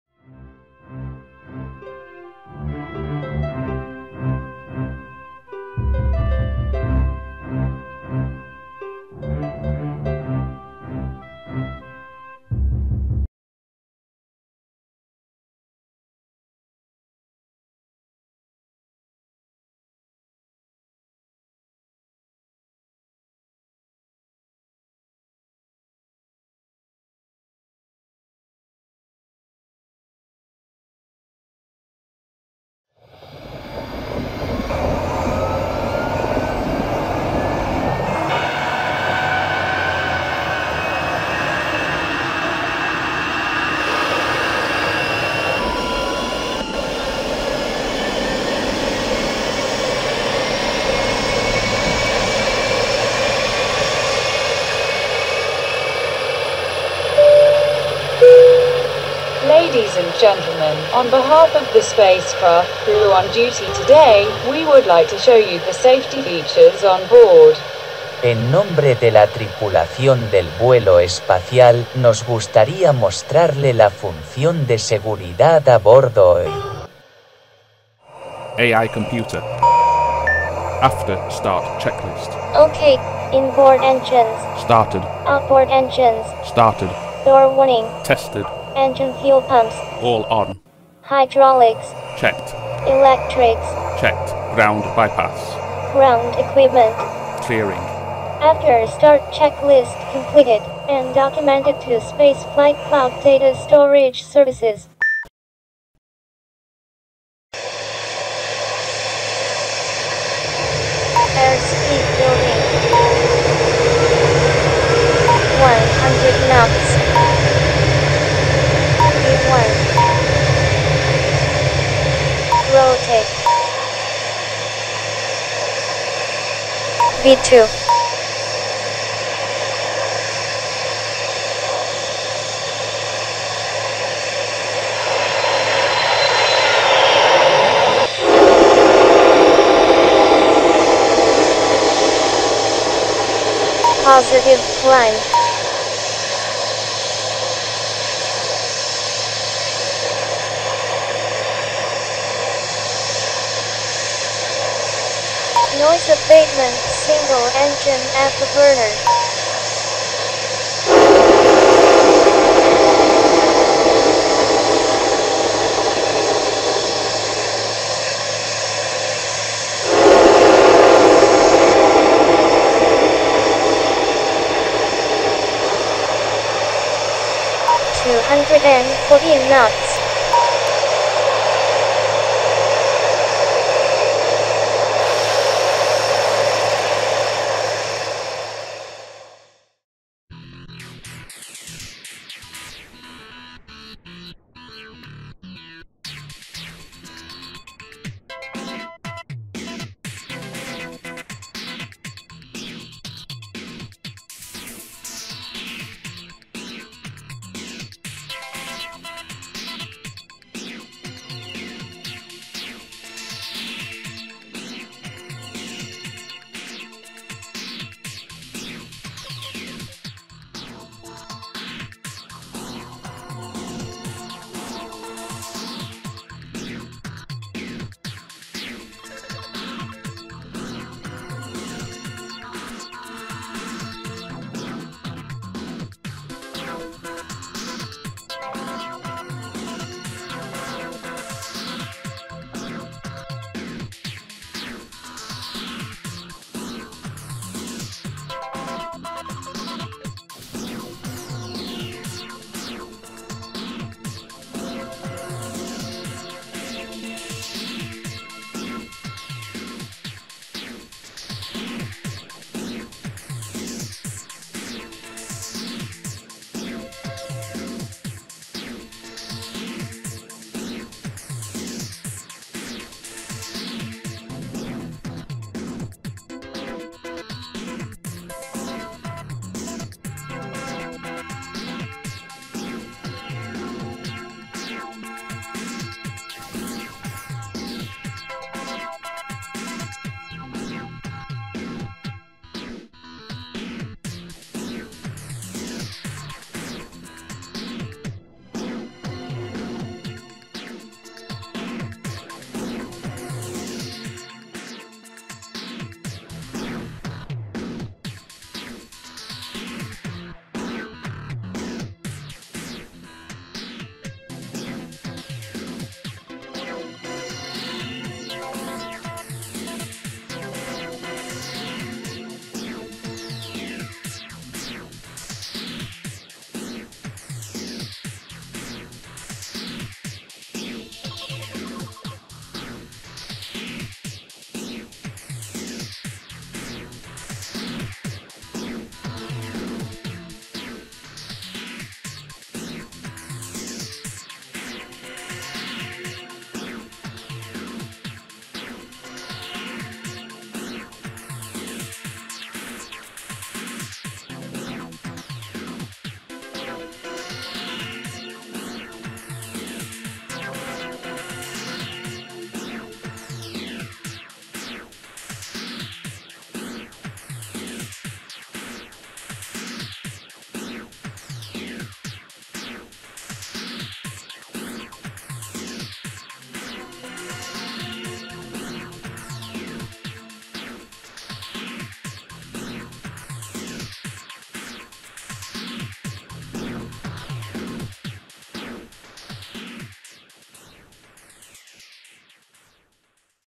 SPACECRAFT TAKEOFF RWY 09 MIAMI International Airport KMIA
SPACE HISTORY:
Yuri Alekseyevich Gagarin was a Soviet Air Forces pilot and cosmonaut was the first human to travel
Credit: NASA/JSC Launch date: April 12, 1981 Astronauts: John W. Young and Robert L. Crippen Takes the viewer through the hours prior to launch of Space Shuttle Columbia, the world's first reusable spaceship, through lift-off;